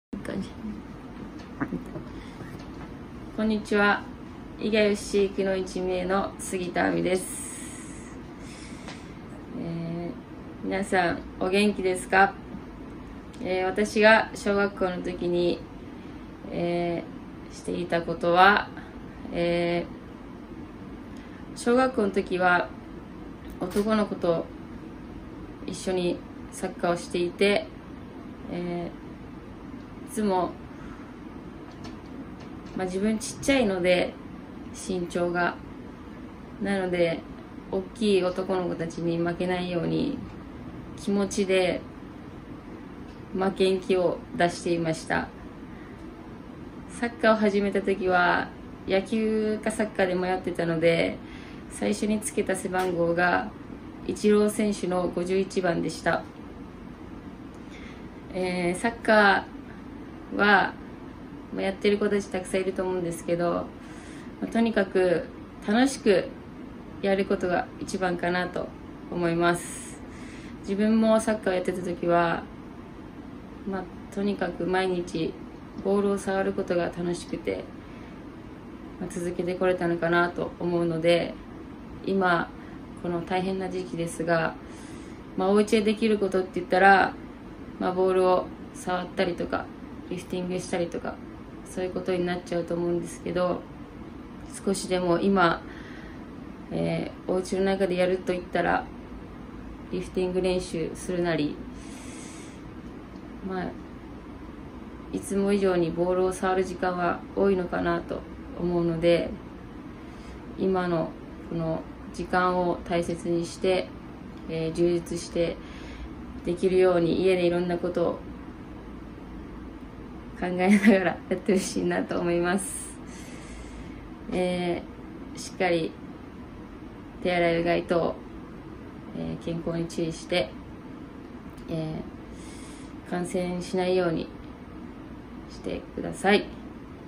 私がサッカー少女だったころ 杉田亜未（伊賀ＦＣくノ一三重）
💌全国のサッカー少女の皆さんへ、
みんなを応援する女子サッカー選手より。
なでしこリーグ所属クラブの選手が『私がサッカー少女だったころ』というテーマについて語ります！

『私がサッカー少女だったころ」の動画の中で、女子サッカー選手たちが教えてくれることはこの５つです！

①どんな子どもだった？
②よくやっていた練習
③夢をかなえるために
④クラブの現状
⑤今、伝えたいこと

全国のサッカー少女の皆さん、大好きなサッカーができず我慢の状況が続いていることと思います。ボールを蹴る喜びを忘れず、夢を諦めずにいてくださいね。

女子サッカー選手たちから皆さんへ向けた『私がサッカー少女だったころ』連載スタートです！